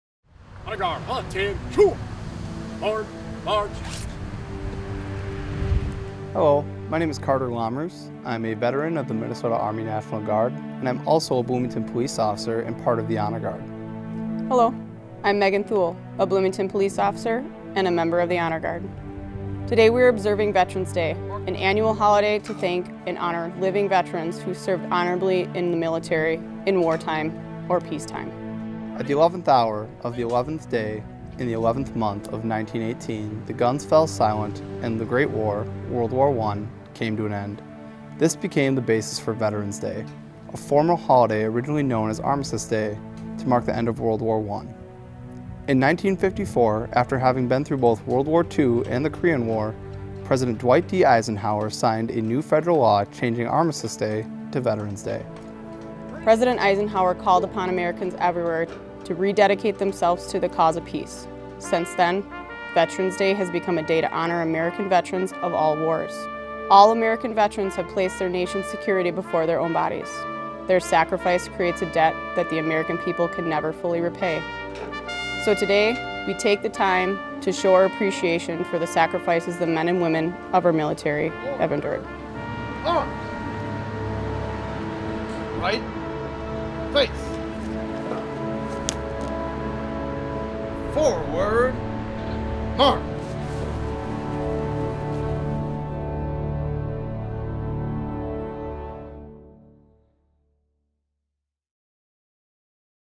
BPD Honor Guard Veterans Day
The Bloomington Police Department Honor Guard shares how Veterans Day became a federal holiday, and why we celebrate it.